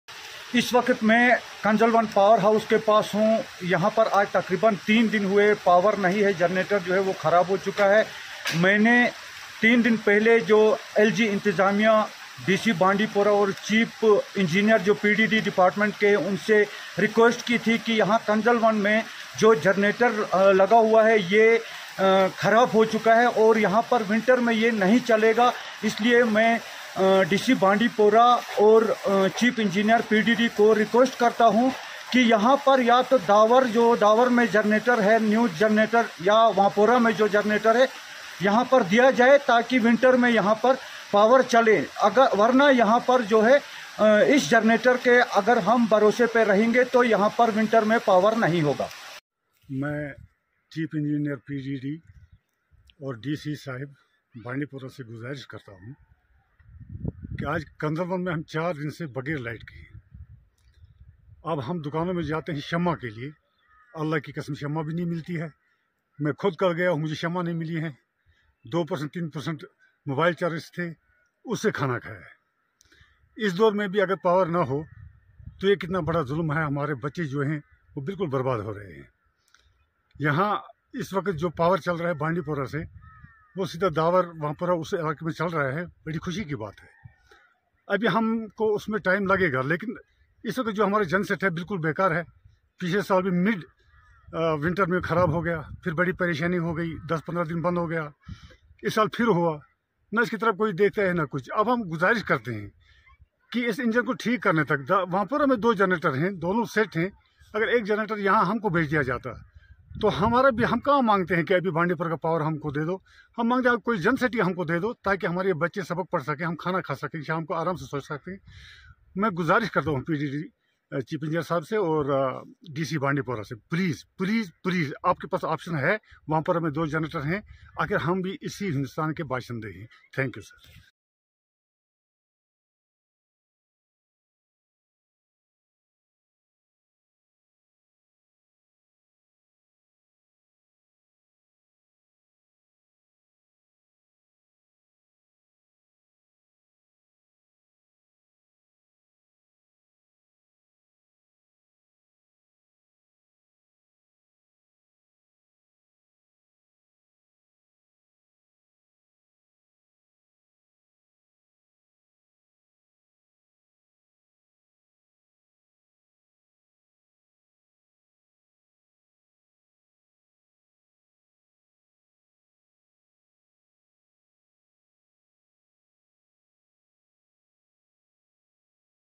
Several villages in Gurez without electricity, Genset develops a snag yet to repair
Several villages in Gurez without electricity from last three day and Gen set develops a snag yet to repair